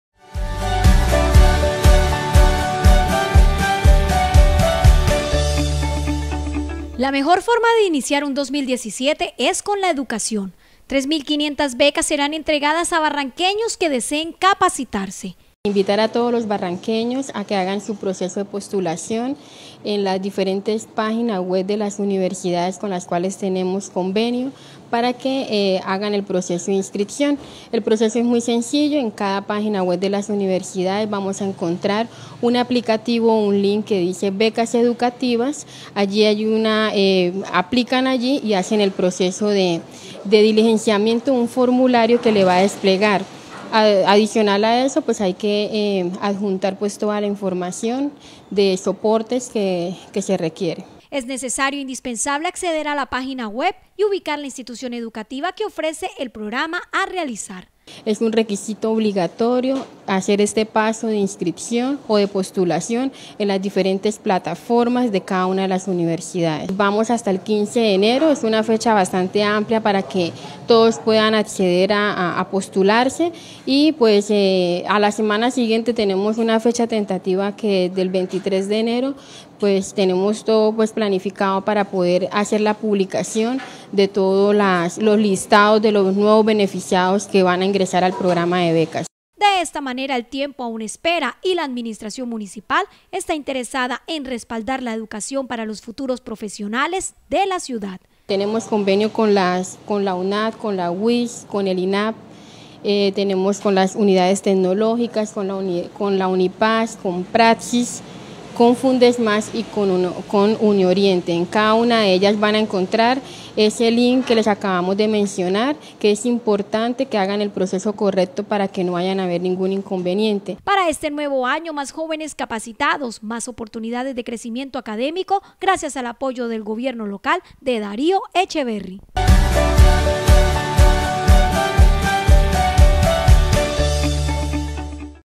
3500 becas serán entregadas a barranqueños que deseen capacitarse.
La mejor forma de iniciar un 2017 es con la educación. Para este nuevo año, habrá más jóvenes capacitados, más oportunidades de crecimiento académico, gracias al apoyo del gobierno de Darío Echeverri.

Es necesario e indispensable acceder a la página web y ubicar la institución educativa que ofrece el programa a realizar. De esta manera, el tiempo aún espera y la administración municipal está interesada en respaldar la educación para los futuros profesionales de la ciudad.